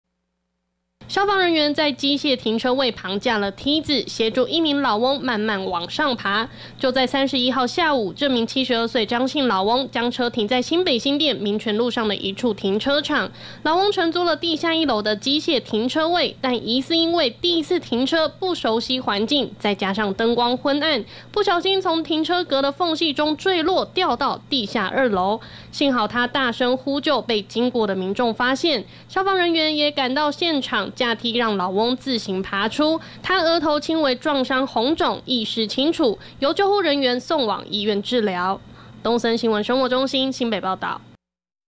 7旬翁從「機械車位縫隙」摔落！ 地下1樓掉地下2樓@newsebc
新北新店一處停車場，31號下午有一名70多歲老翁，掉入機械停車位的縫隙中，從地下1樓摔落到地下2樓，警消也趕緊到場，架梯子協助老翁脫困。